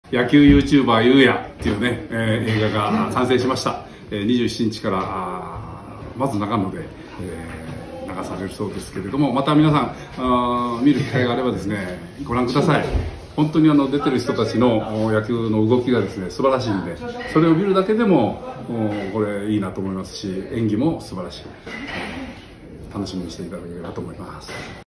田尾さんに「野球ユーチューバー有矢」を応援していただいてます
10/27より長野千石劇場で絶賛公開中
10/27、28は舞台挨拶へのご来場ありがとうございました。